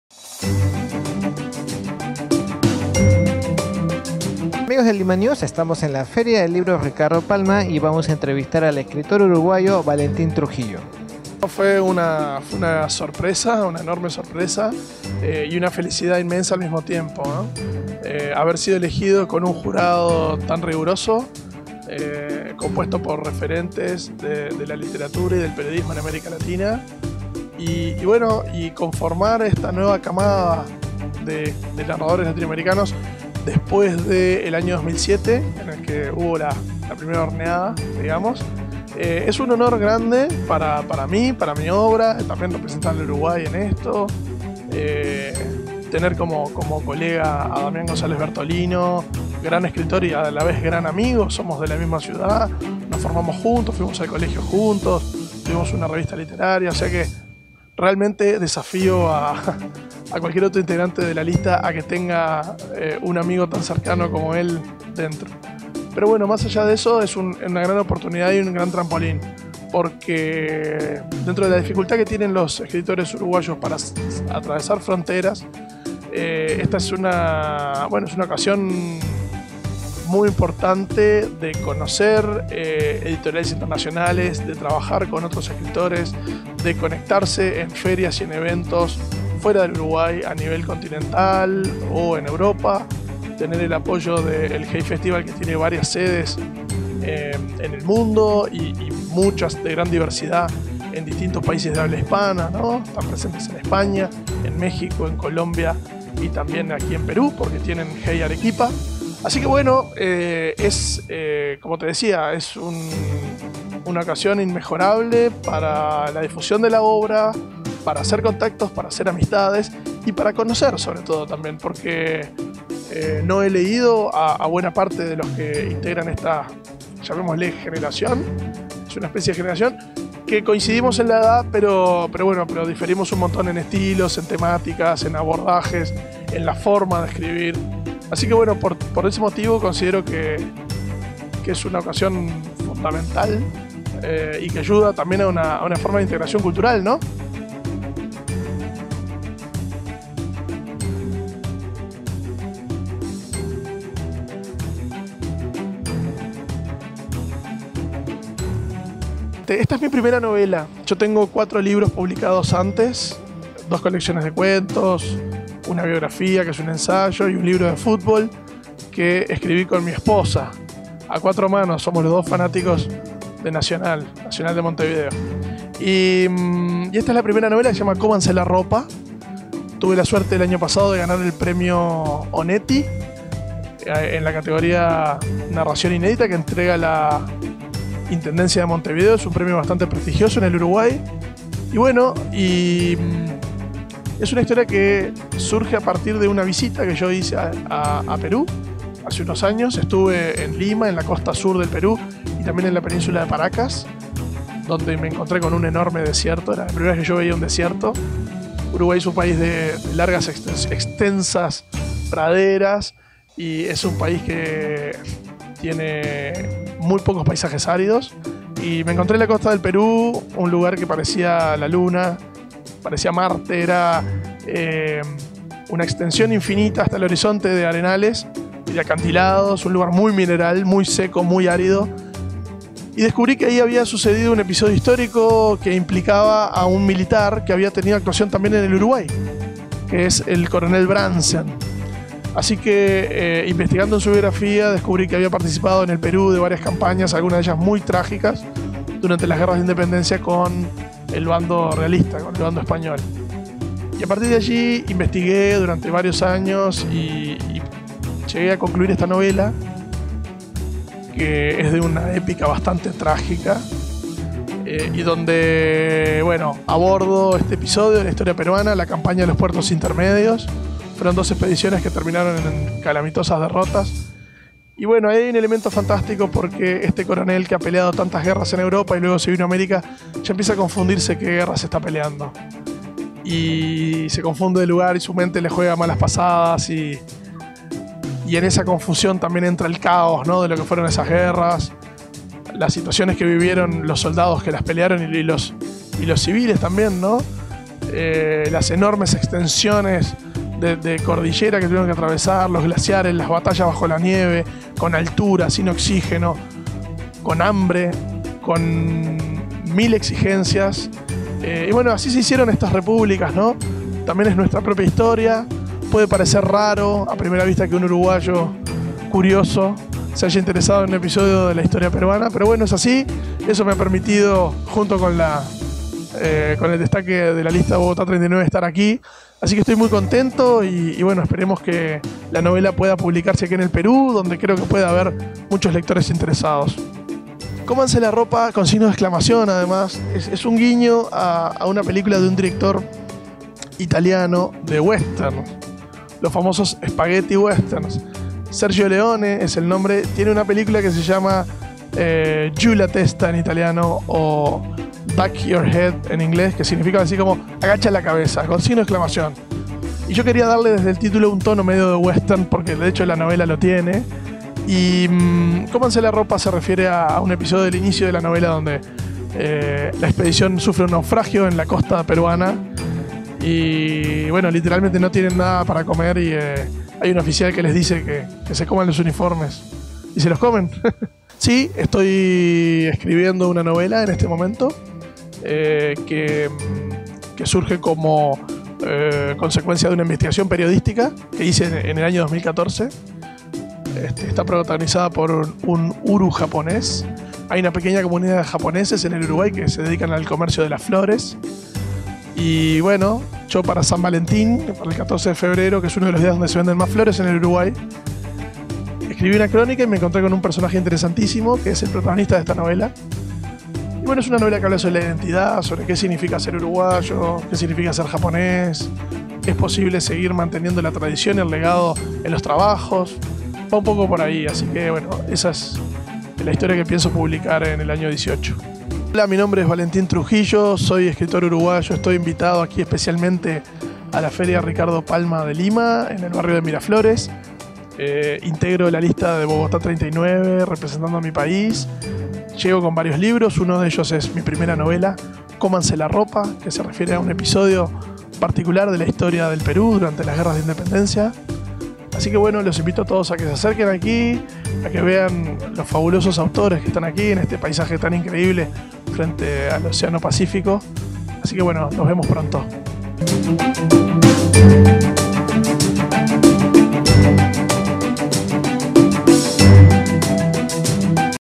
LimaNews - Entrevista Valentín Trujillo
Valentín Trujillo es un escritor uruguayo que estudió cine en Cinemateca Uruguaya. Se graduó de profesor de Lengua y Literatura en Maldonado. Estudió periodismo en la Universidad Católica del Uruguay.
En 2007 obtuvo el Premio Nacional de Narrativa Juan José Morosoli por su libro de cuentos Jaula de costillas, publicado por editorial Banda Oriental.
En 2013 publicó su segunda antología de cuentos, Entre jíbaros
(Estuario), que obtuvo mención de honor en los Premios de Literatura del Ministerio de Cultura. Ese mismo año publicó junto a su esposa, la periodista Elena Risso, el libro Nacional 88, historia íntima de una hazaña, un ensayo deportivo sobre el Club Nacional de Football, campeón de la Copa Libertadores de América y de la Copa Intercontinental en 1988.
En 2016 obtuvo el Premio Onetti en la categoría Narrativa, por su novela ¡Cómanse la ropa!, a publicarse en 2017. También en 2016 un cuento suyo integró la antología 13 que cuentan, publicado por Banda Oriental. En 2017 editó, a través de Ediciones B, Real de Azúa. Una biografía intelectual, con la que obtuvo el Premio Bartolomé Hidalgo 2017.

¡VISITA NUESTRAS PÁGINAS!